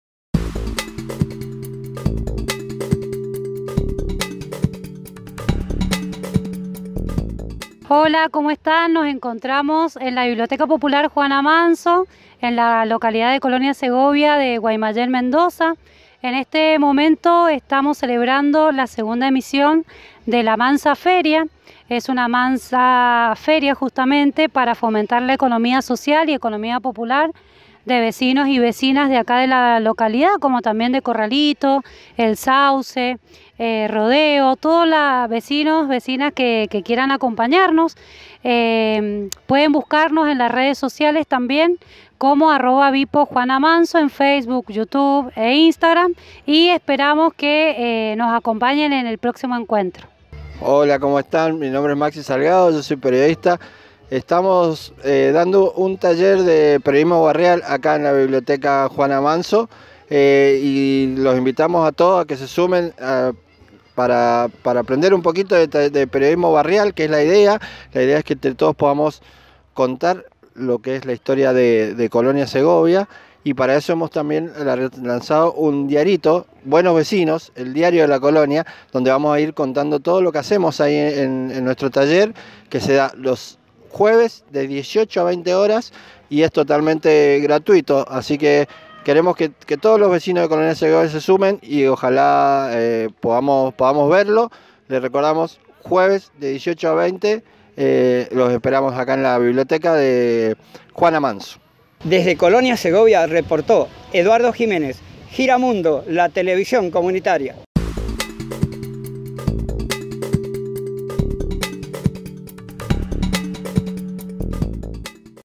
LA MANSA FERIA en Colonia Segovia
El sábado 19 de marzo de 2022 se llevó adelante la segunda edición de La Mansa Feria, organizada por la Biblioteca Popular Juana Manso con el objetivo de fortalecer la economía popular de las y los vecinos de Colonia Segovia, Guaymallén, Mza.
Este informe nos llega gracias al gran compromiso de Eduardo Giménez.